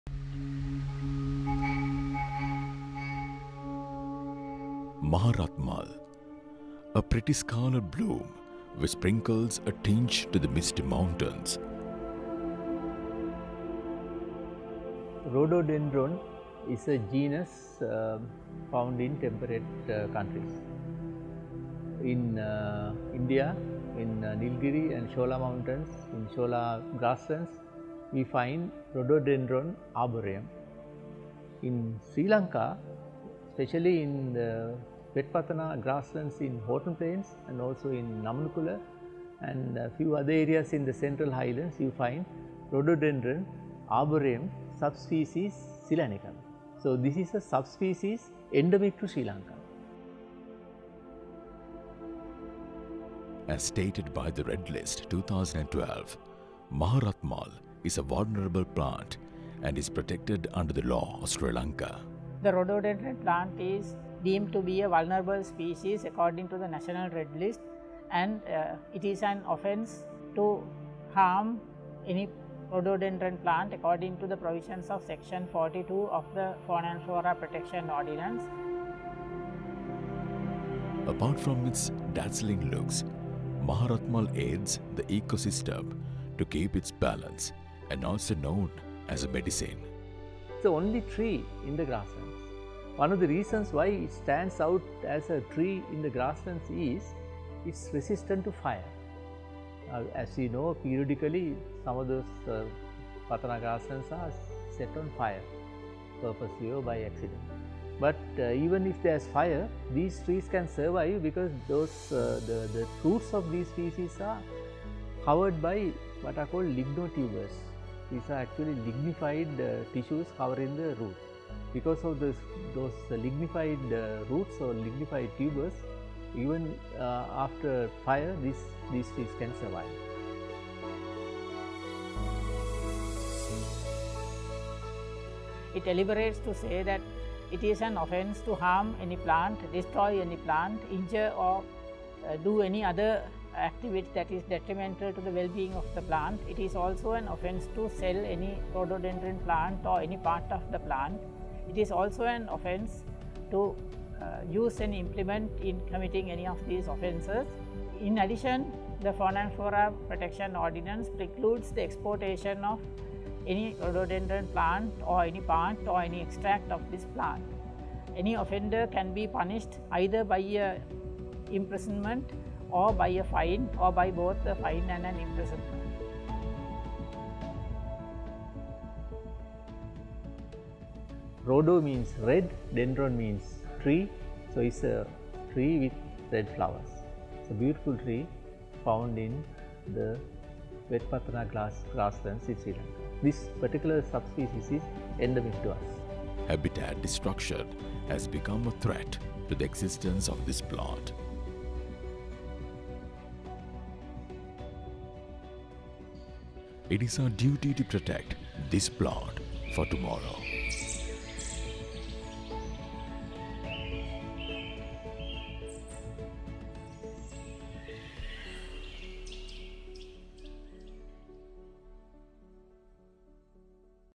Maha Rathmal (මහ රත්මල්) - Rhododendron arboreum zeylanicum
Maharathmal, Rhododendron arboreum zeylanicum, a pretty scarlet bloom which sprinkles a tinge to the misty mountains, belongs to genus Ericaceae is an endemic subspecies of R. arboreum.